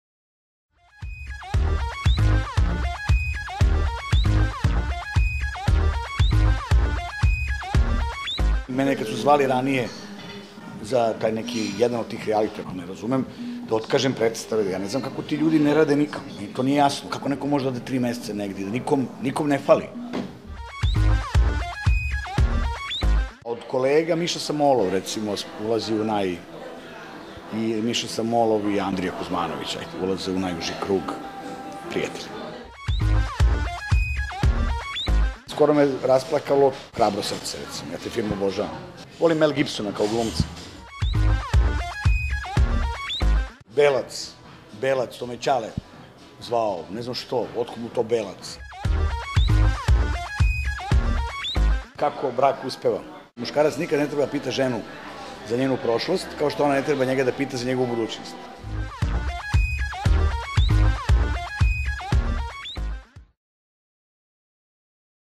Marko Živić | 60 sekundi za MONDO TV
Marko Živić popularni glumac za MONDO otkriva da li bi ušao u rijaliti, koji je recept za uspešan brak i koji ga je film rasplakao.